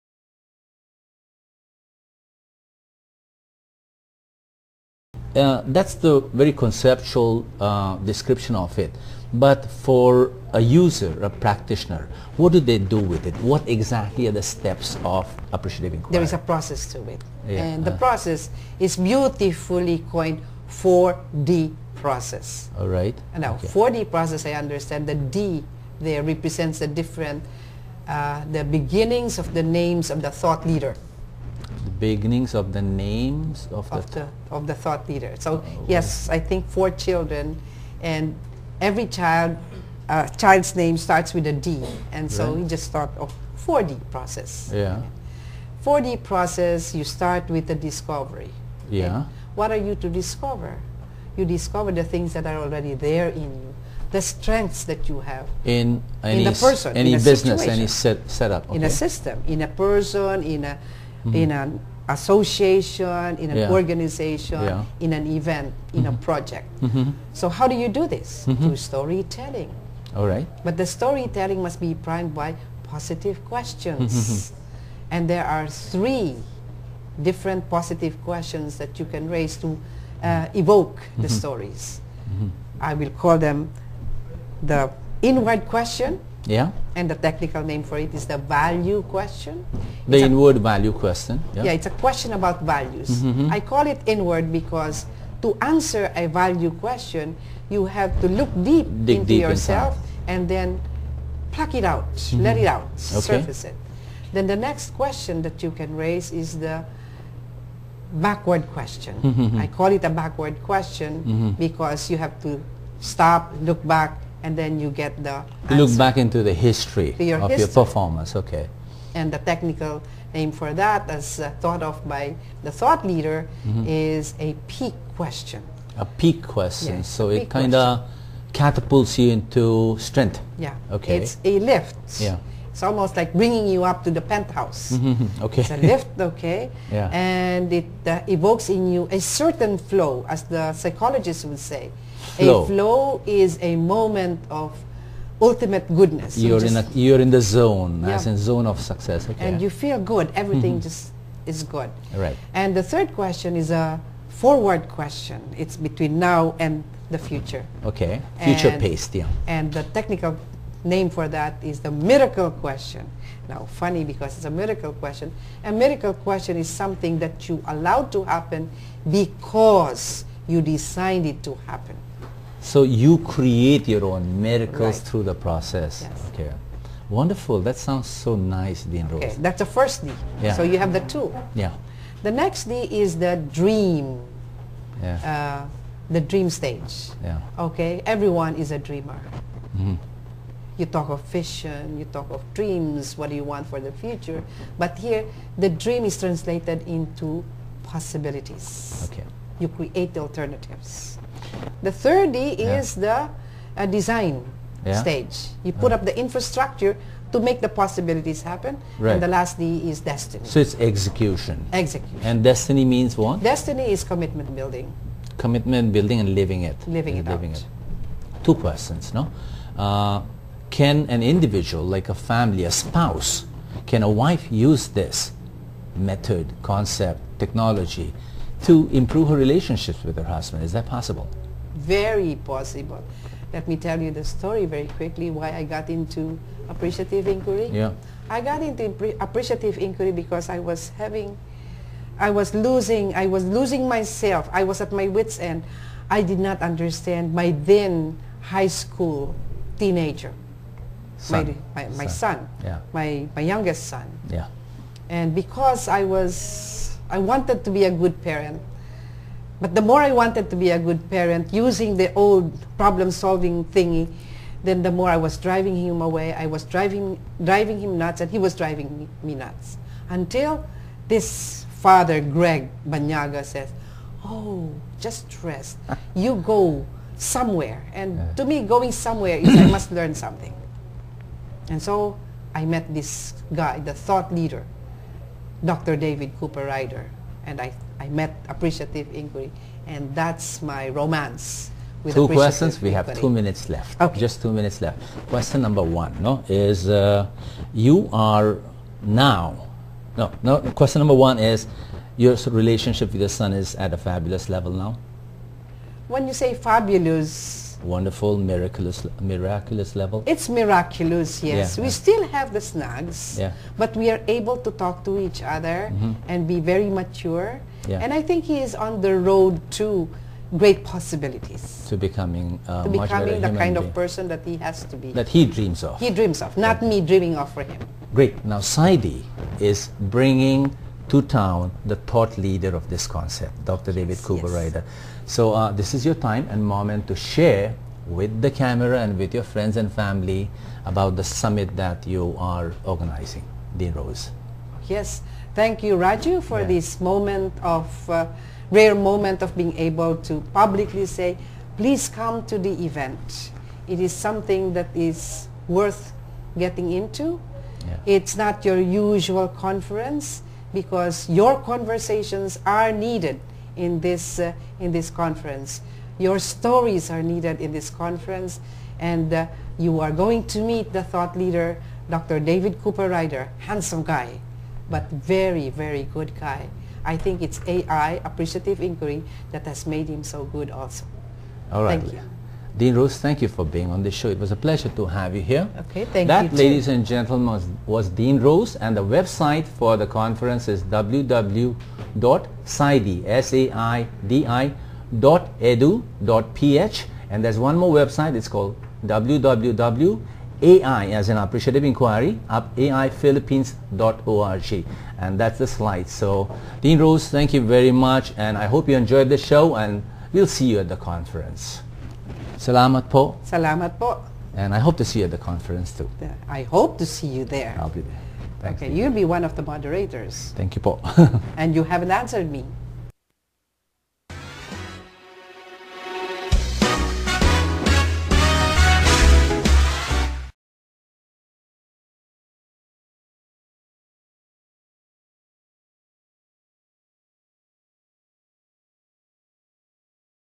Appreciative Inquiry :ExIn110610 Appreciative Inquiry Part 2
Raju Mandhyan I have, for years, been a devotee, a do-er and a dealer of "Appreciative Inquiry." To me, "to appreciate is to love, to forgive and to nurture." To inquire is to be curious, to wonder and want to create, construct and build. These principles and values want to live and thrive in the core of my being. It is not a method..., a process or an approach but a way of life for me.

Appreciative Inquiry through the words of Dean Rose Fuentes of the Southeast Asian Interdisciplinary Institute. She tells host, Raju Mandhyan, about how Appreciative Inquiry can be employed for Organisational Development in businesses. . In the end there is an invitation for everyone to participate in the 1st Asian Appreciative Inquiry Summit to be held on November 29-30 at the Shangrila, Edsa with the presence of Dr. David Cooperrider.